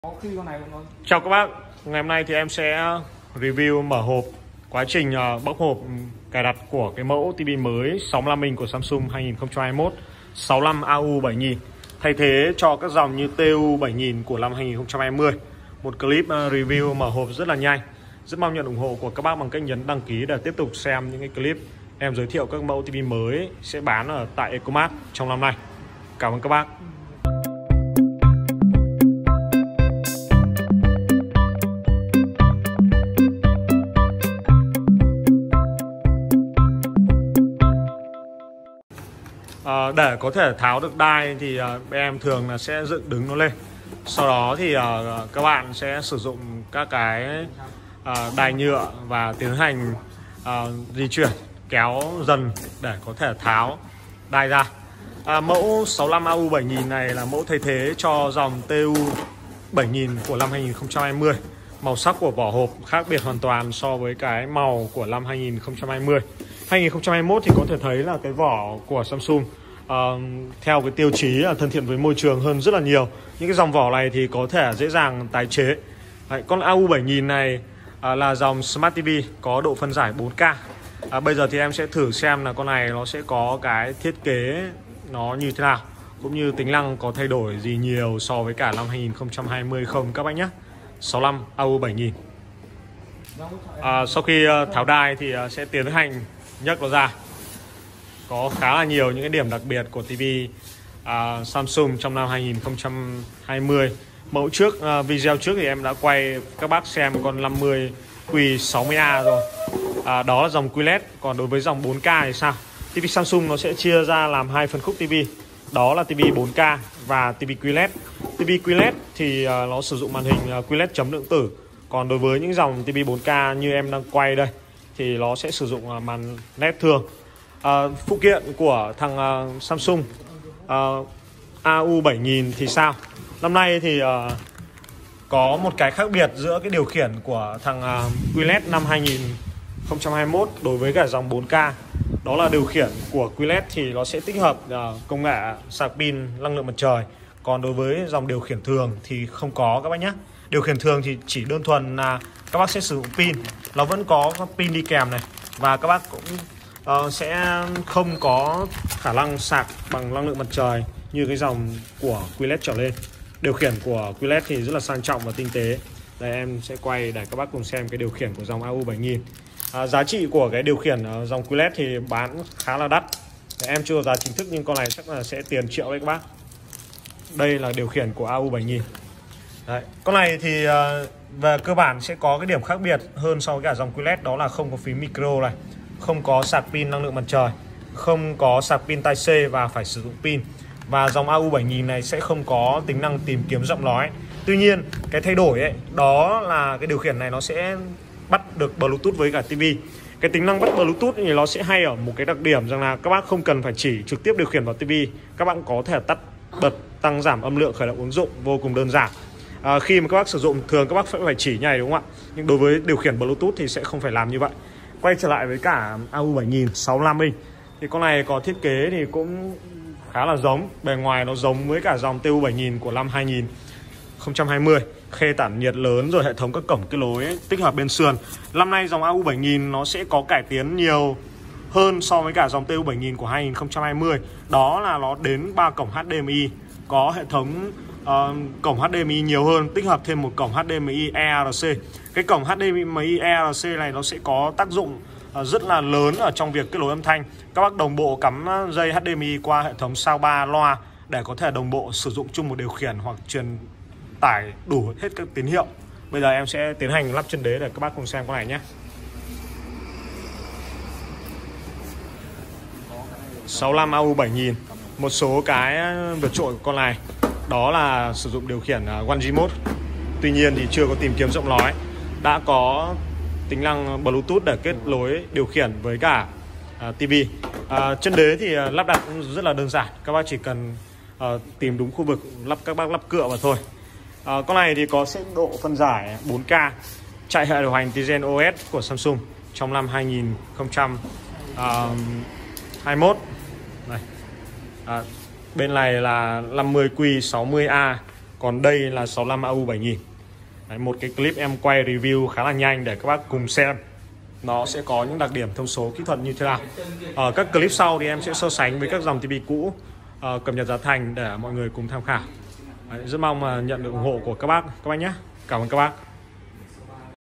Mở hộp TV Samsung 65AU7000 màn 65inch 4K Smart New 2021 Onremote
Mở hộp đánh giá Tivi mới Samsung 65AU7000 mới 2021